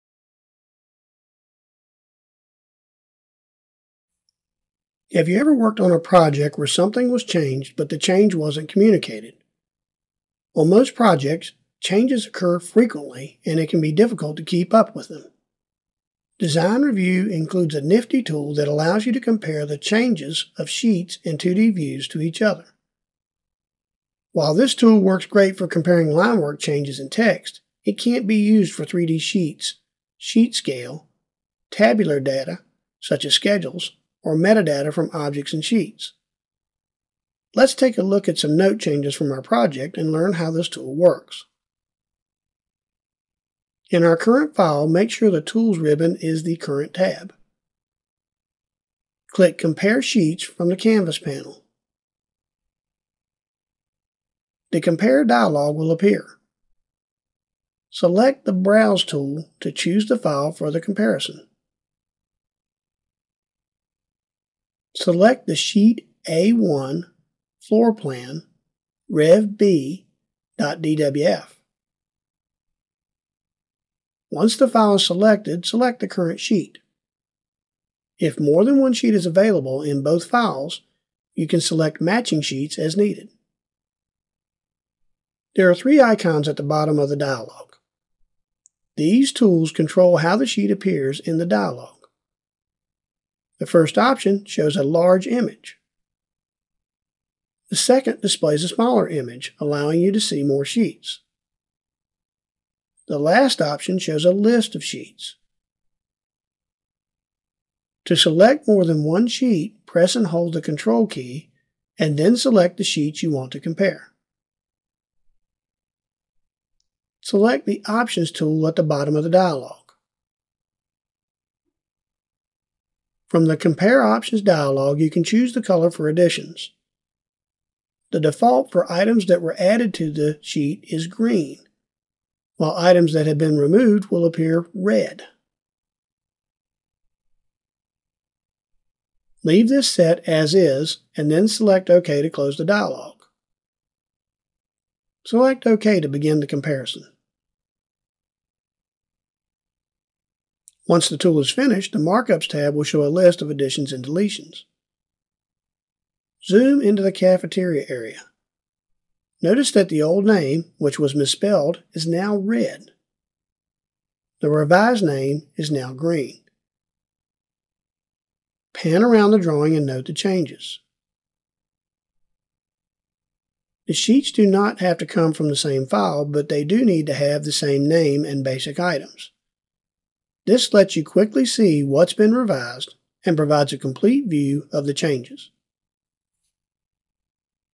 Comparing 2D Views
Comparing differences between 2D Views in Autodesk Design Review 2013.